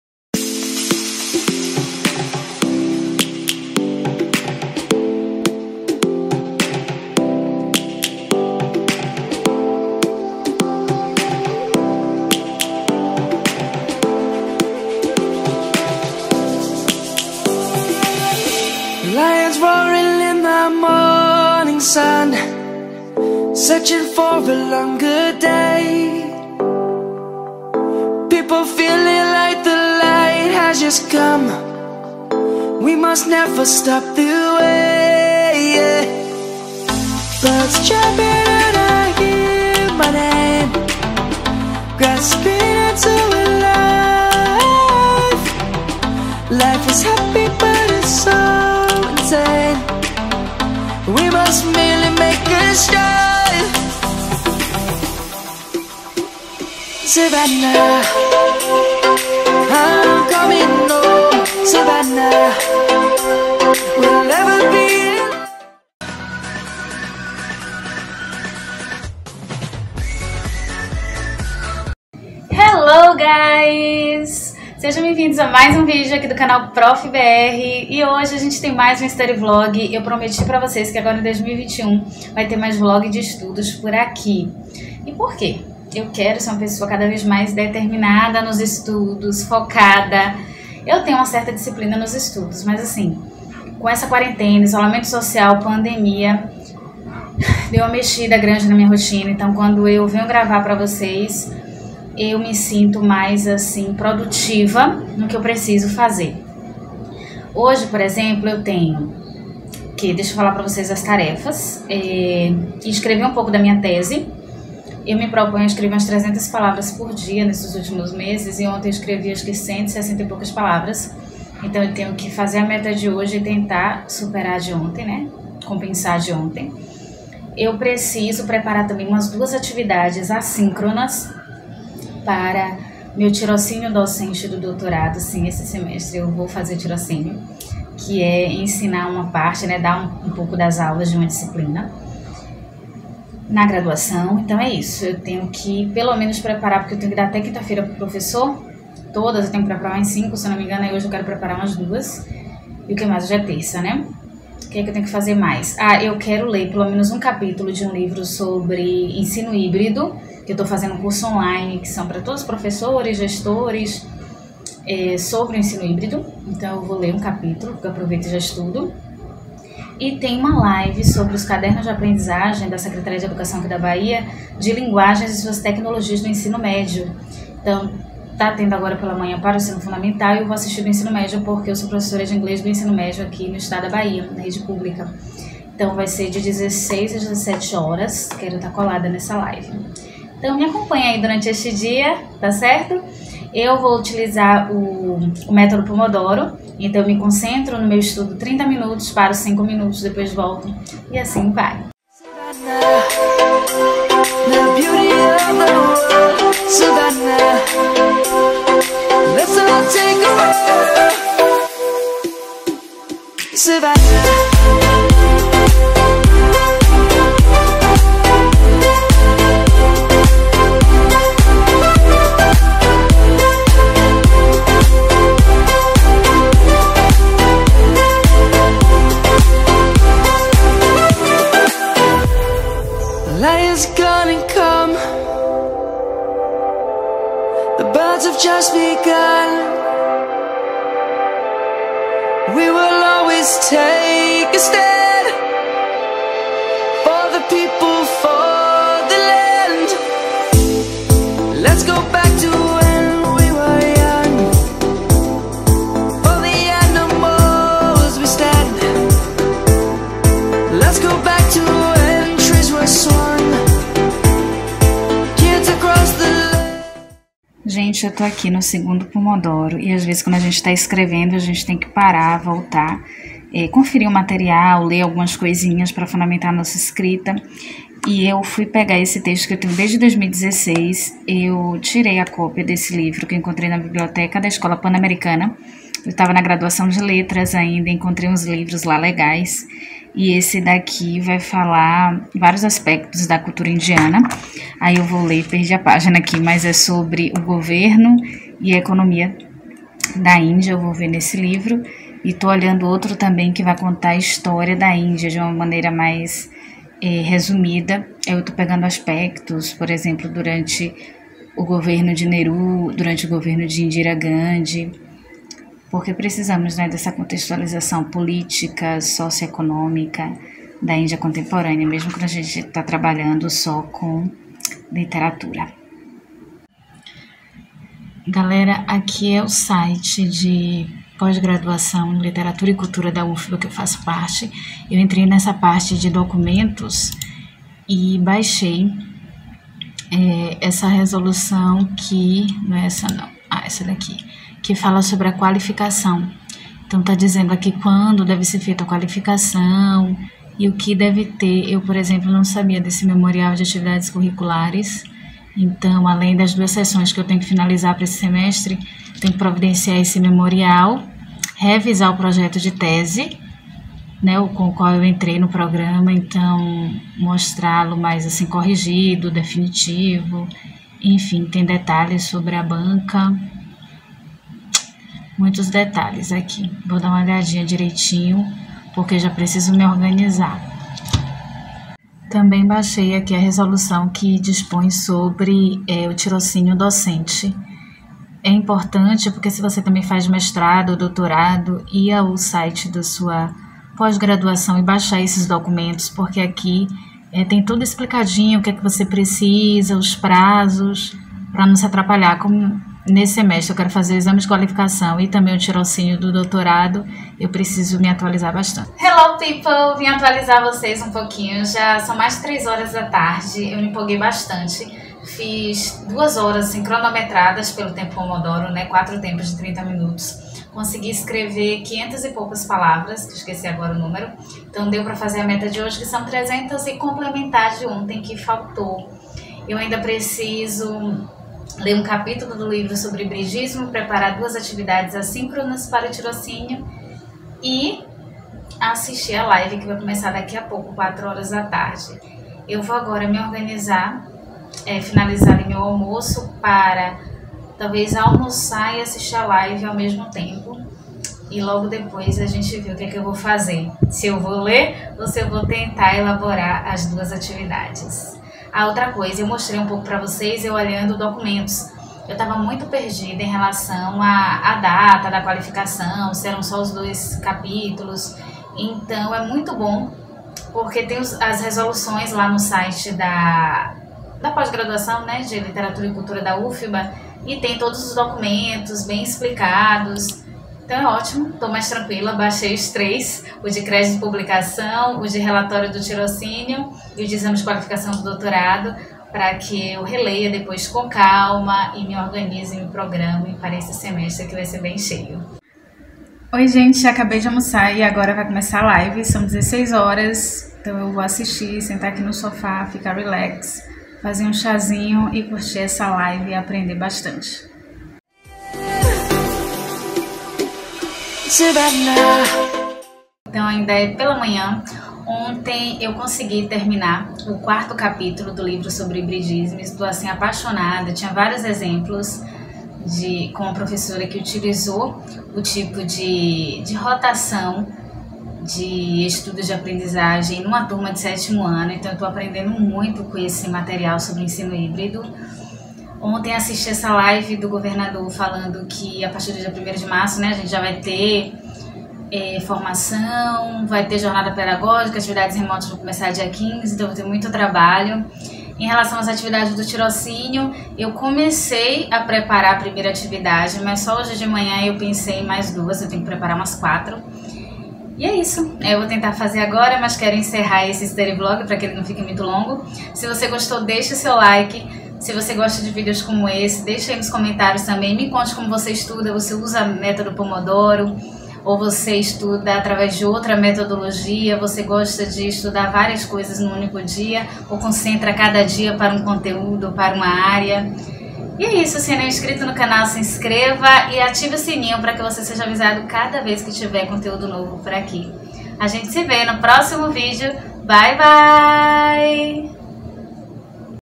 STUDY VLOG: Tirocínio do doutorado + leituras
Neste vídeo você vai me acompanhar na minha rotina enquanto doutoranda e professora. É preciso estudar sempre mesmo quando não estamos muito motivados!
Meu instagram: @prof.br